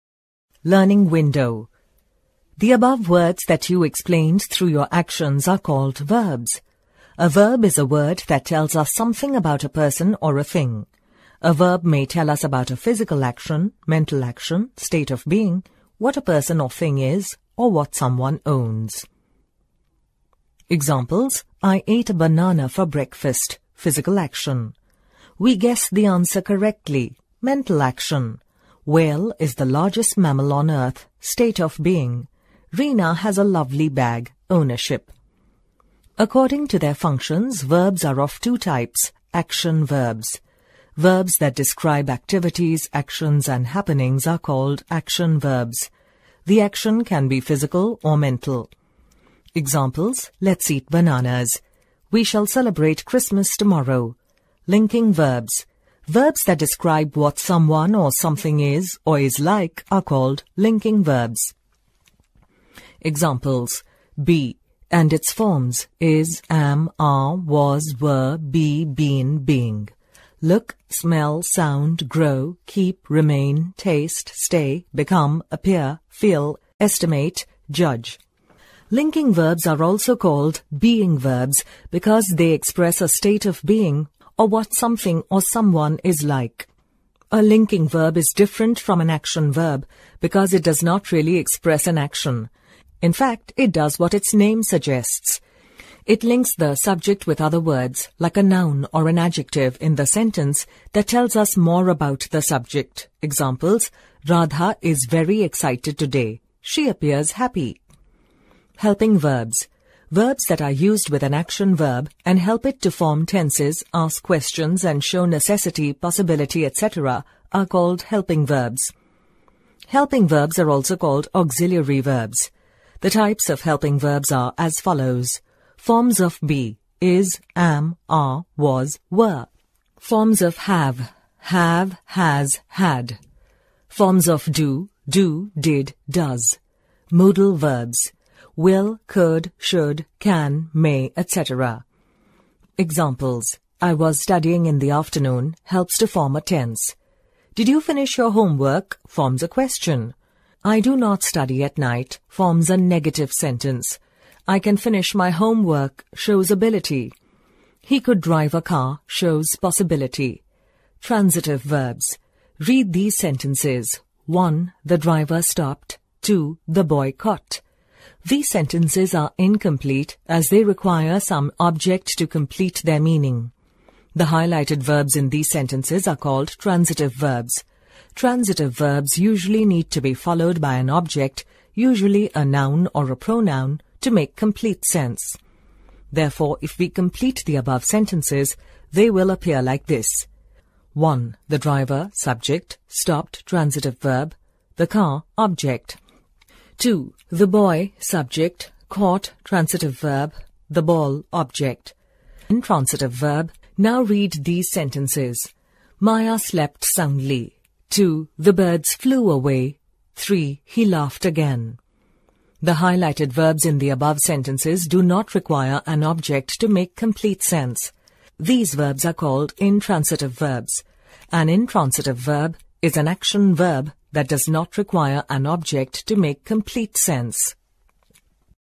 Verbs | Action Verbs | Linking Verbs | Helping Verbs | Transitive Verbs | Grammar for Class 5
• A verb is a word that tells us something about a person or a thing. A verb may tell us about a physical or mental action, what a person or thing is, or what someone owns.
• Linking verbs link the subject with other words in the sentence that tell us more about the subject.
• Helping verbs are used with the main verbs in the sentence to express tenses, to form negatives, to ask questions and to show possibility, ability, permission, etc.
• A transitive verb is a verb that usually needs to be followed by an object (a noun or a pronoun) to make complete sense.
• An intransitive verb is a verb that does not require an object to make complete sense.
Grammar for Class 5 @Blueprint Digital